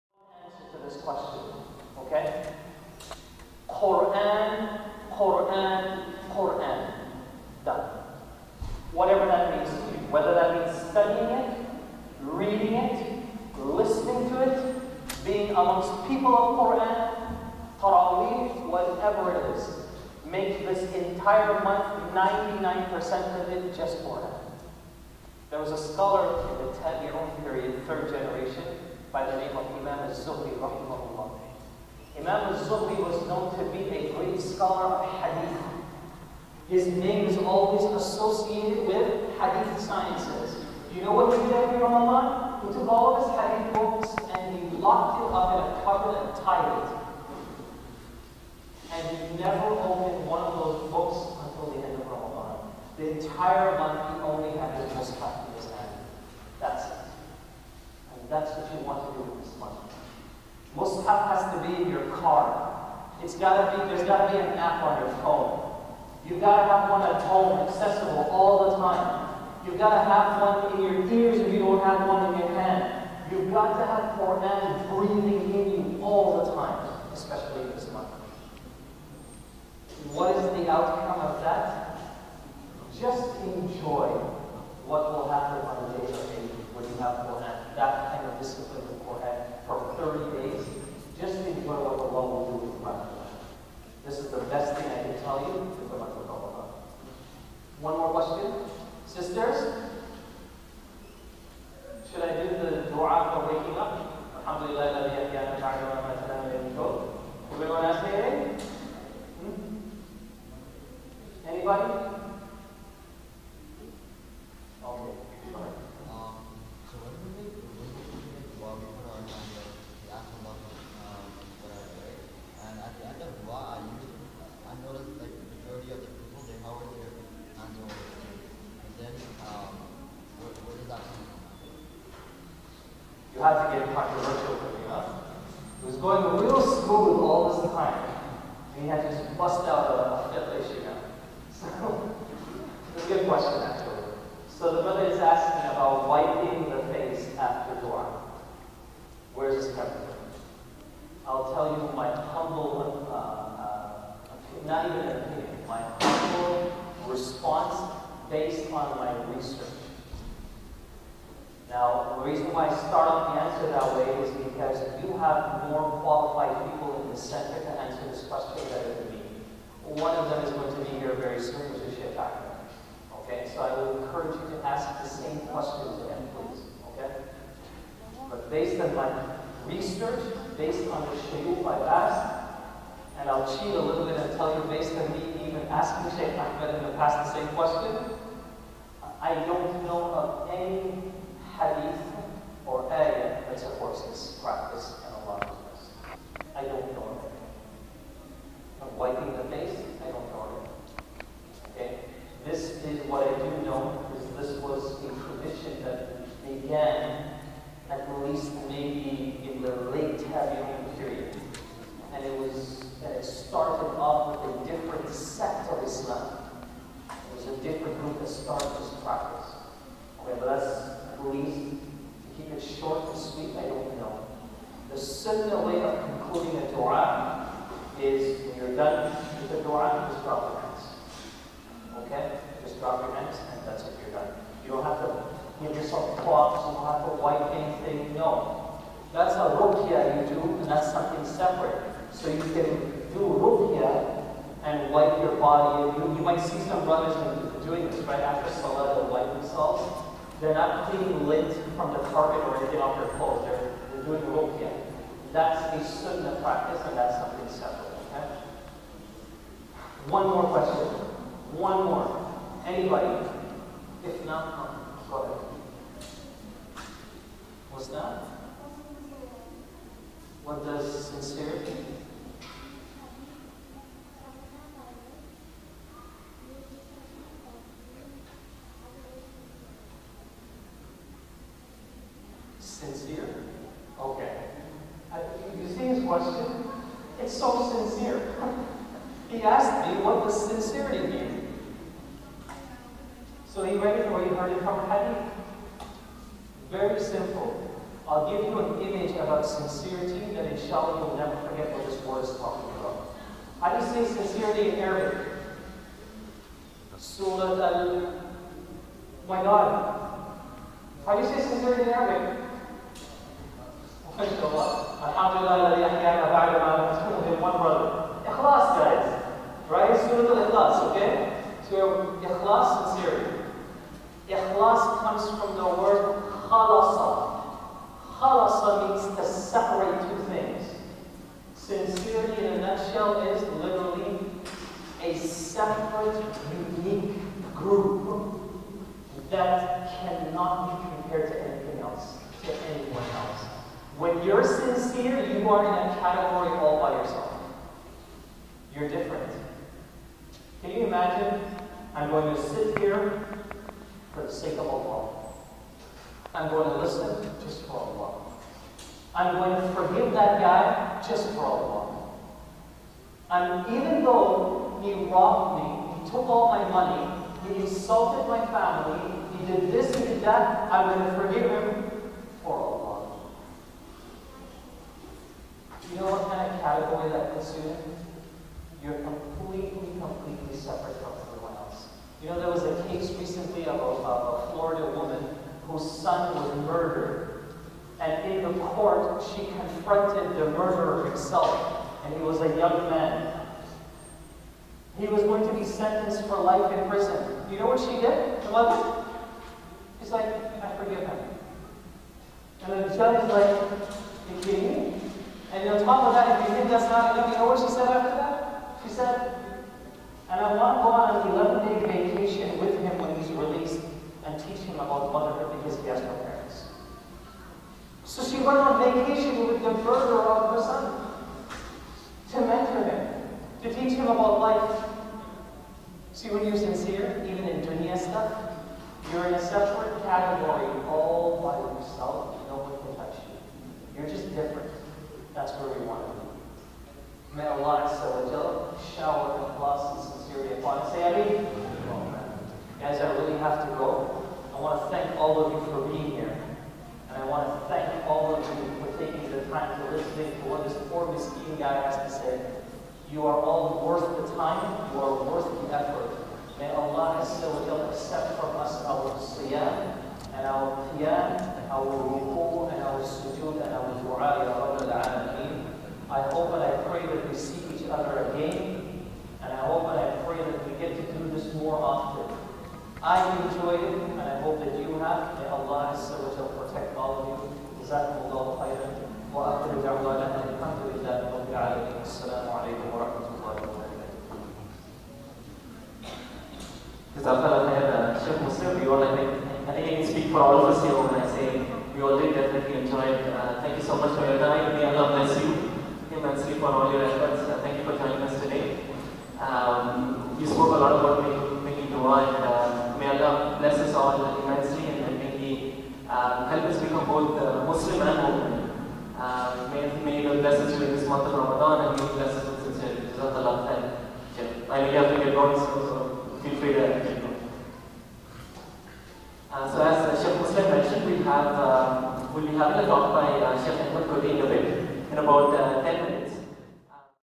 HiMY SYeD - Shaikh Musleh Khan, Special Youth Session Q&A, Islamic Institute of Toronto, July 4 2015
HiMY SYeD -- Shaikh Musleh Khan, Special Youth Session, Q&A, Islamic Institute of Toronto, Saturday July 4 2015